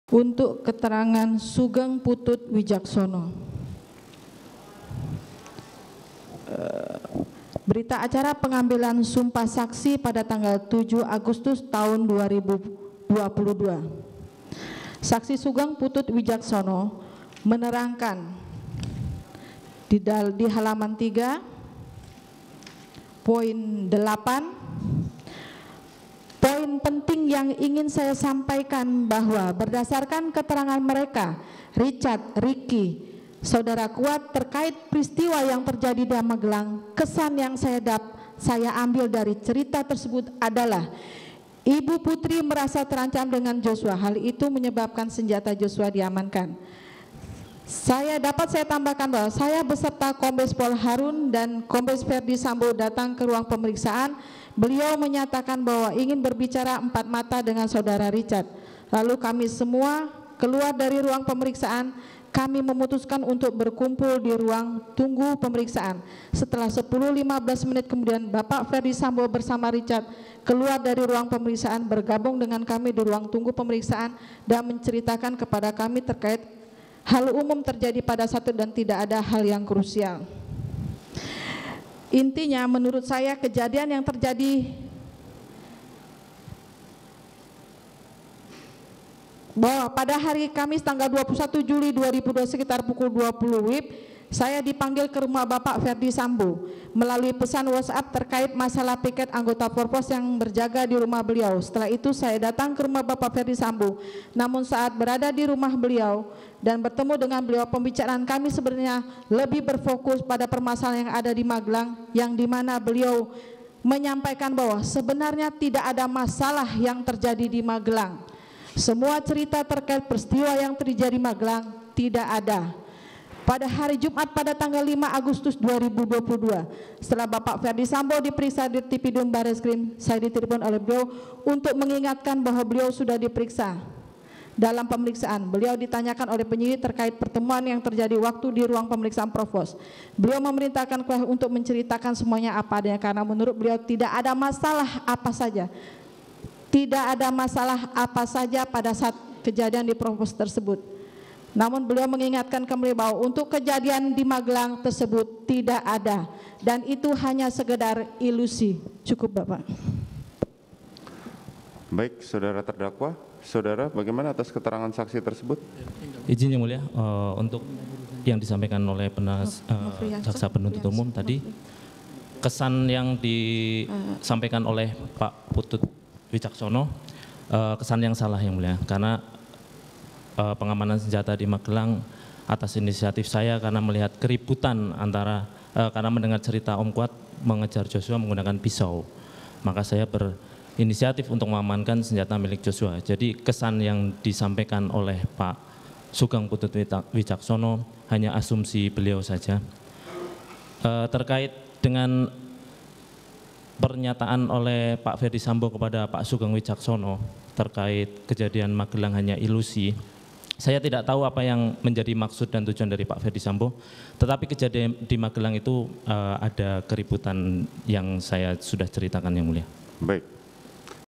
Dalam BAP, Saksi Sugeng Ungkap Ferdy Sambo Sebut Tidak Ada Peristiwa di Magelang
JAKARTA, KOMPAS.TV - Jaksa Penuntut Umum membacakan berita acara pemeriksaan (BAP) milik saksi Sugeng Putut Wicaksono yang tidak hadir di persidangan hari ini (09/01/2023).

Dalam BAP tersebut, Sugeng mengatakan dari keterangan Eliezer, Ricky, Kuat Maruf terkait peristiwa yang terjadi di Magelang bahwa Ibu Putri Candrawathi merasa terancam dengan Yosua, hal itu yang membuat senjata Yosua diamankan.

Sugeng juga mengatakan Ferdy Sambo sebut tidak ada peristiwa di Magelang atau ilusi.

Baca Juga Jaksa Penuntut Umum Bacakan BAP Saksi yang Tidak Bisa Hadir di Persidangan di

Aktifkan juga lonceng supaya kamu dapat notifikasi video terbaru dari KompasTV.